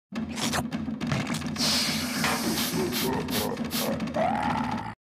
Nickelodeon - Brushing Teeth ID (1996, HQ)
Nickelodeon's NFT account posted this in reply to my tweet saying "Post More Bumpers!" (i think)

Also known as the "Toothbrush ID", or the "Brushing Teeth ID".

Designed by Drucker Motion Pictures in 1996.